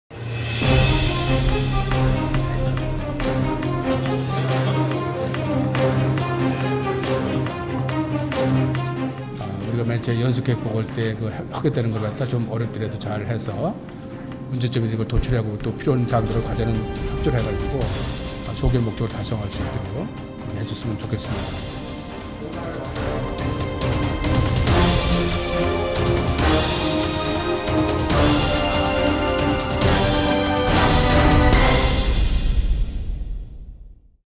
박인용 장관, 국민안전처 을지연습 종합상황실 현장 순시
박인용 국민안전처 장관은 8월 23일 국민안전처 을지연습 종합상황실을 방문해 을지연습 중인 직원들을 격려하는 등 현장을 순시하였습니다.
국가비상사태를 대비한 을지연습이 내실있는 훈련이 될 수 있도록 국민안전처가 최선을 다하겠습니다.